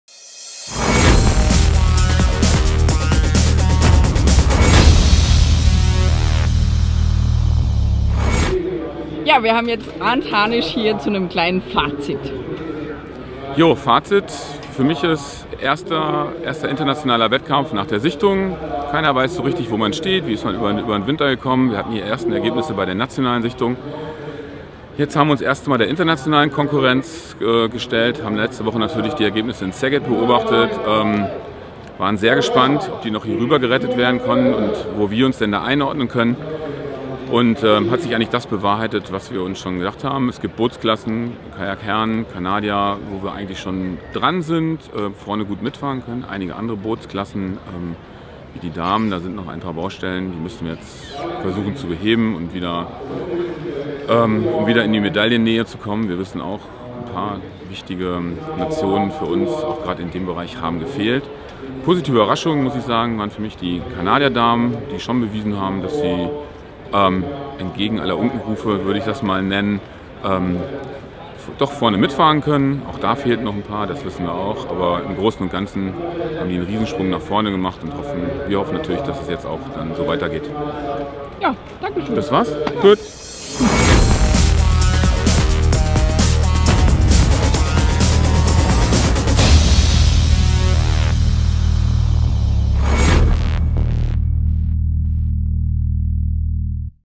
WC Duisburg Fazit Arndt Hanisch
Der leitende DKV-Bundestrainer Kanu-Rennsport zieht ein gemischtes Fazit und benennt Stärken und Schwächen des deutschen Teams.